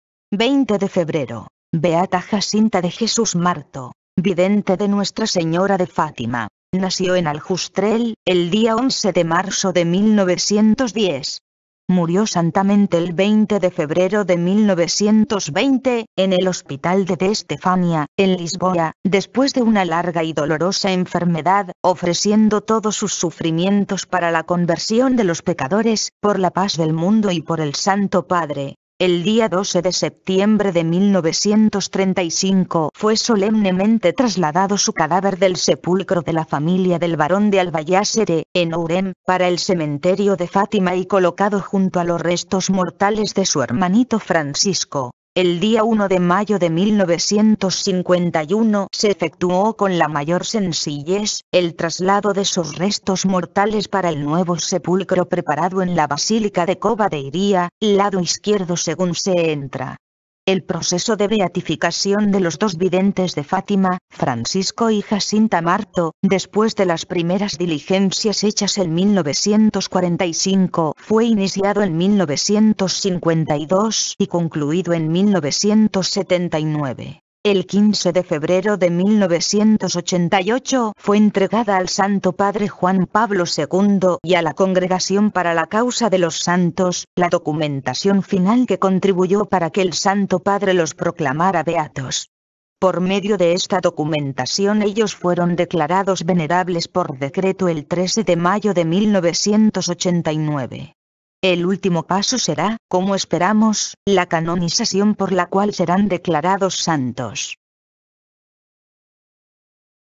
BEATA JACINTA DE JESÚS MARTO,20 de febrero,Vidas Ejemplares
20 de febrero

Vidente de Nuestra Señora de Fátima

Nació en Aljustrel, el día 11 de Marzo de 1910. Murió santamente el 20 de Febrero de 1920, en el Hospital de "D. Estefânia", en Lisboa, después de una larga y dolorosa enfermedad, ofreciendo todos sus sufrimientos para la conversión de los pecadores, por la paz del mundo y por el Santo Padre.

El día 12 de Septiembre de 1935 fue solemnemente trasladado su cadáver del sepulcro de la familia del Barón de Alvaiázere, en Ourém, para el cementerio de Fátima y colocado junto a los restos mortales de su hermanito Francisco.

El día 1 de Mayo de 1951 se efectuó con la mayor sencillez, el traslado de sus restos mortales para el nuevo sepulcro preparado en la basílica de Cova de Iria, (lado izquierdo según se entra). El proceso de beatificación de los dos videntes de Fátima, Francisco y Jacinta Marto, después de las primeras diligencias hechas en 1945 fue iniciado en 1952 y concluido en 1979.

El 15 de Febrero de 1988 fue entregada al Santo Padre Juan Pablo II y a la Congregación para la causa de los Santos, la documentación final que contribuyo para que el Santo Padre los proclamara "beatos". Por medio de esta documentación ellos fueron declarados "venerables" por decreto el 13 de Mayo de 1989. El último paso será, como esperamos, la canonización por la cual serán declarados "santos".